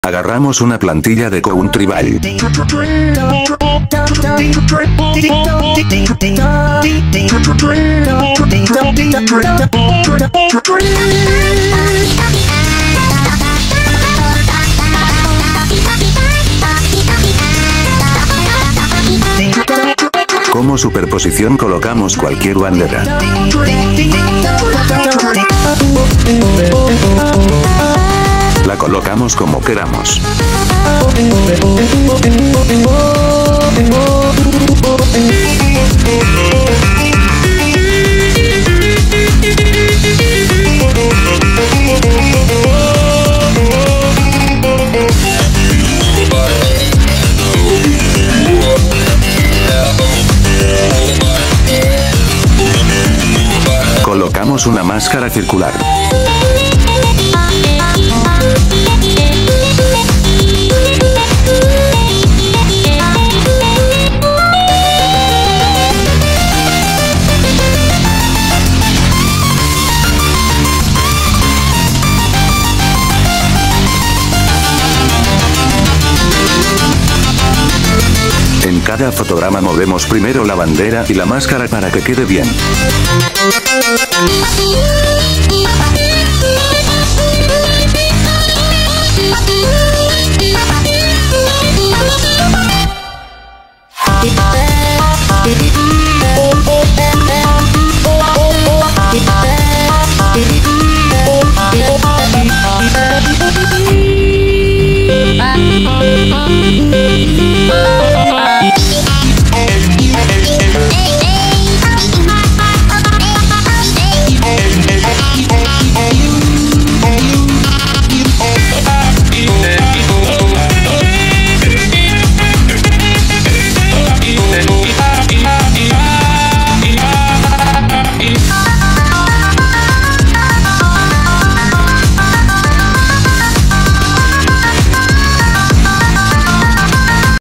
Cómo animar banderas de countryballs con máscaras en capcut (con comentario)
Obviamente agregas los ojos en la countryball después
top formas más épicas de robar xd